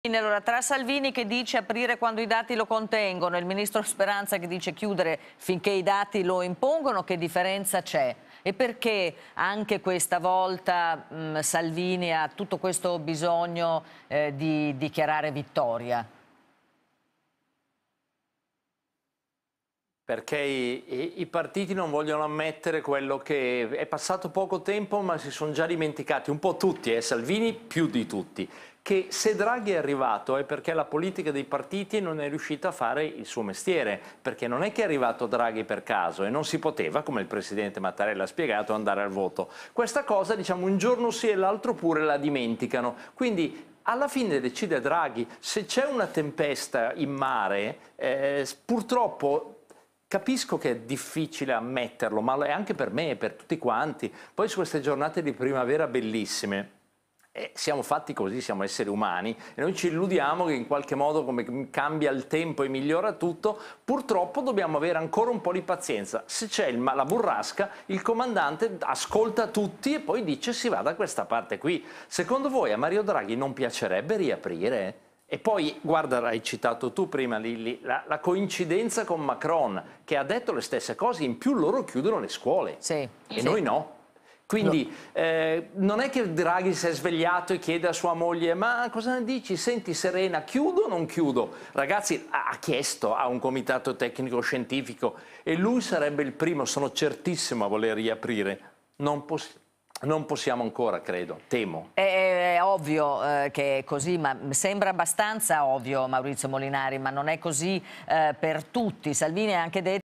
Covid, Severgnini: "Salvini si è dimenticato che Draghi è arrivato perchè la politica ha fallito"
Beppe Severgnini commenta le dichiarazioni di Matteo Salvini dopo l'approvazione del nuovo decreto Covid di aprile: "Salvini si è dimenticato che Draghi è arrivato perchè la politica ha fallito"